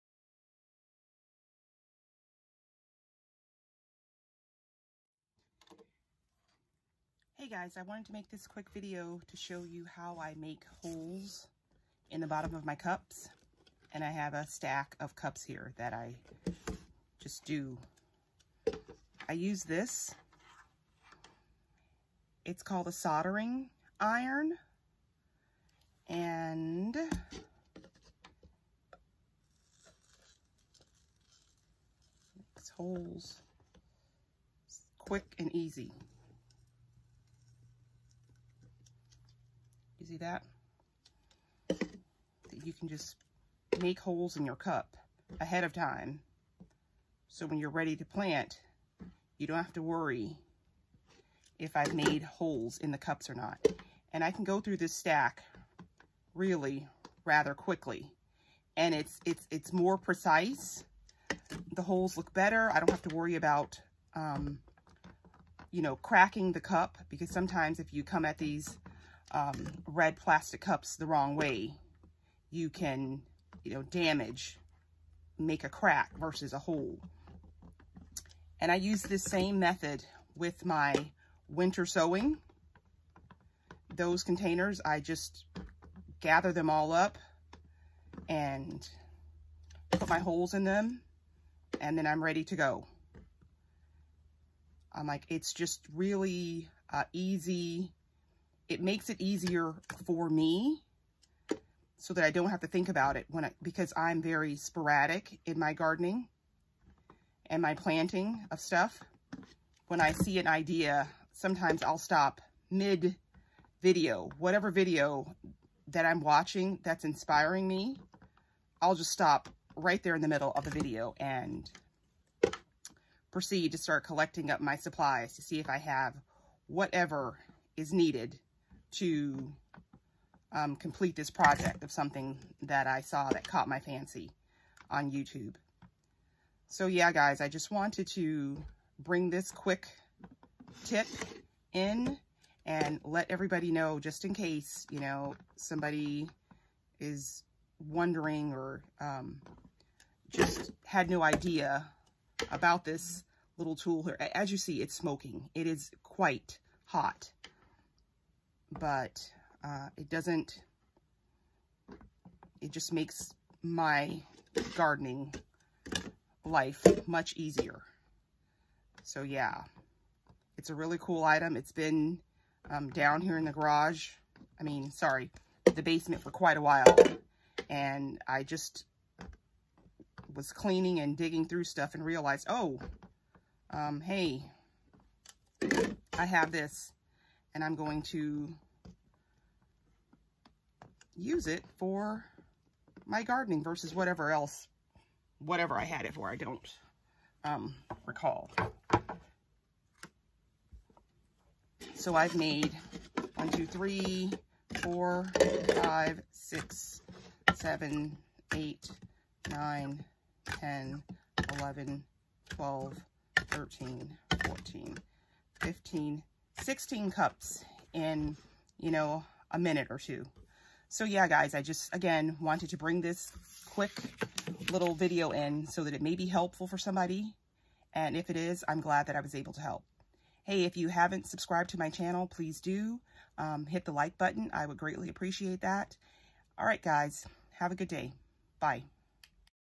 Using a soldering iron
How I quickly make holes in my cups and containers before planting.